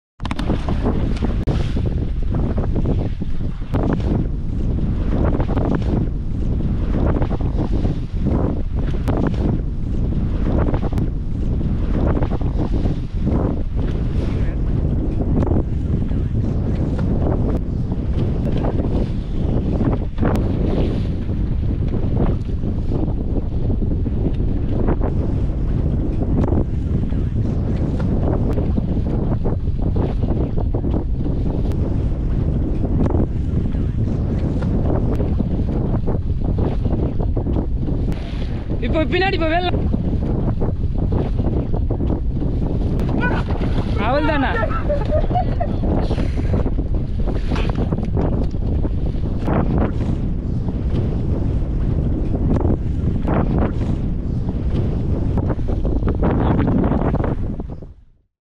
Deep Sea Fishing Videos
We Caught King Fish in the Deep Sea